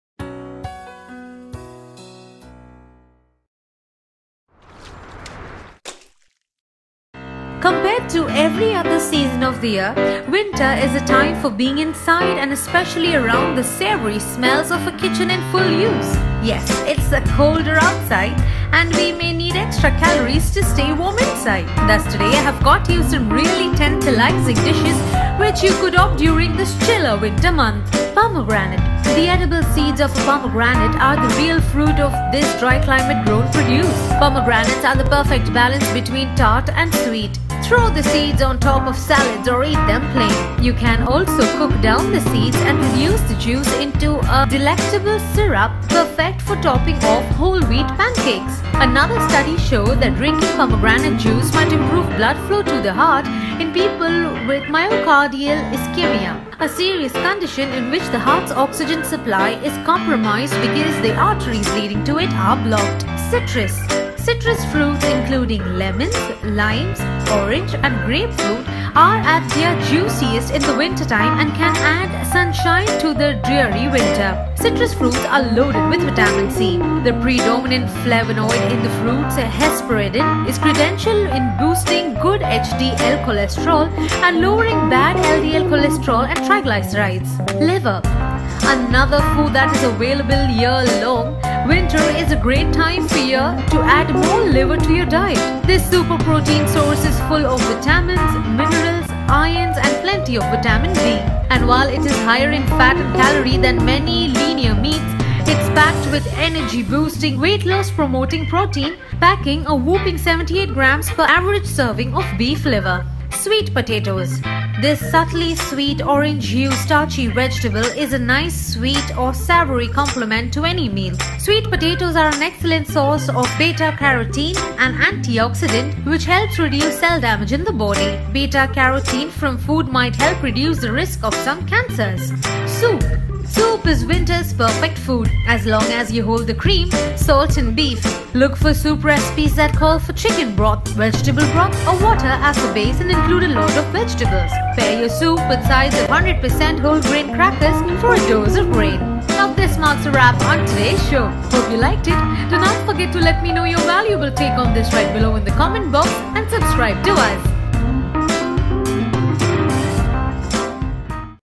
Craziest Things To Eat This Season | The Short Cuts Food
Compared to every other season of the year, winter is a time for being inside, and especially around the savory smells of a kitchen in full use. Yes, it is colder outside, and we may need extra calories to stay warm.